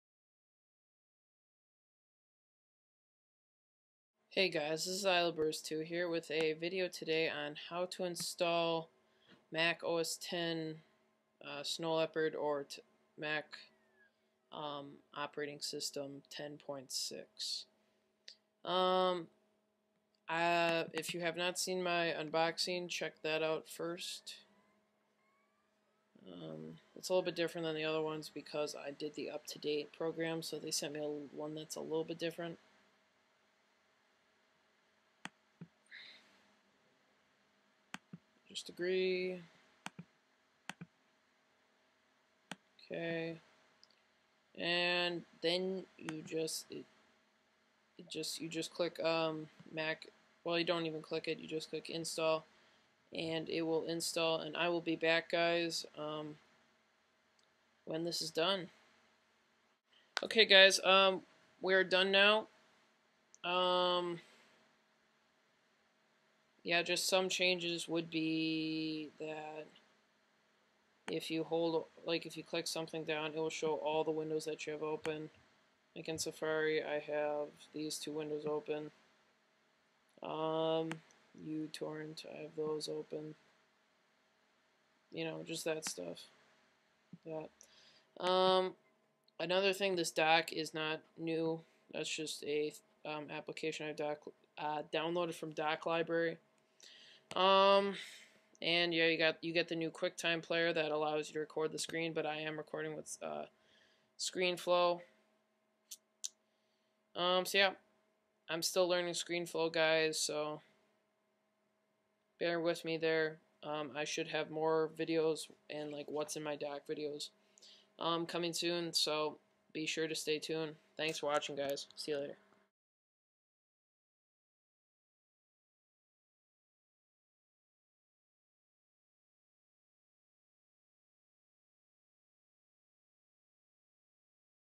How to Install Mac OS X Snow Leopard
How to install Apple's newest operating system, Mac OS 10.6. It is very easy. Also if you want a Lockerz.com invite let me know through a PM and I will be more than happy to send you one.